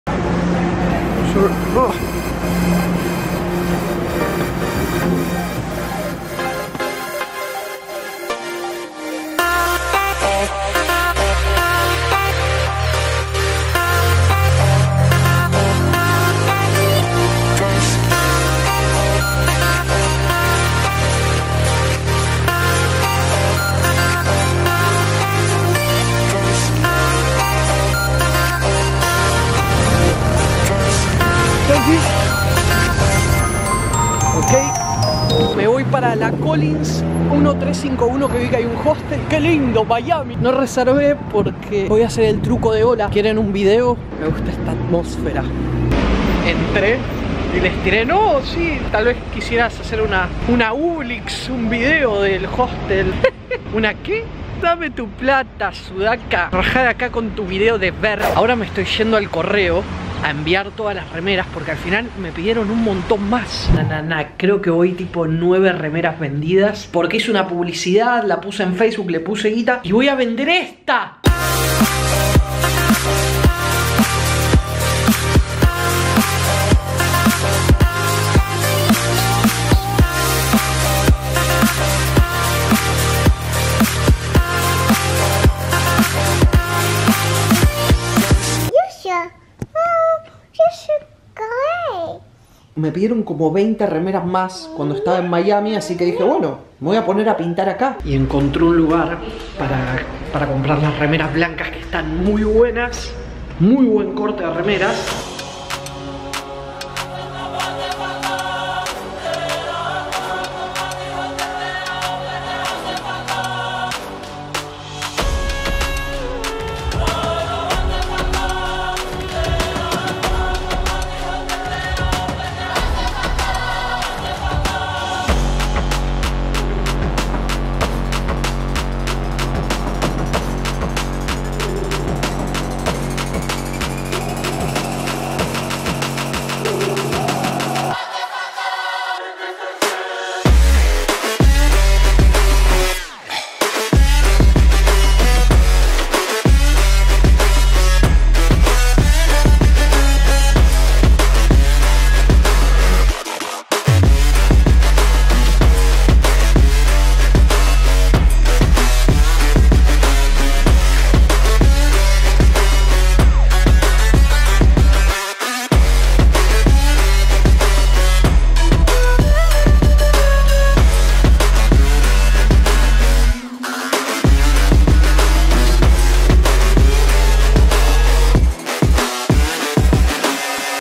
Danzoneando en Miami
Producción sorpresa y foto shoot con nuevos amigos en South Beach. Grandes momentos.

Filmado con Canon G7X.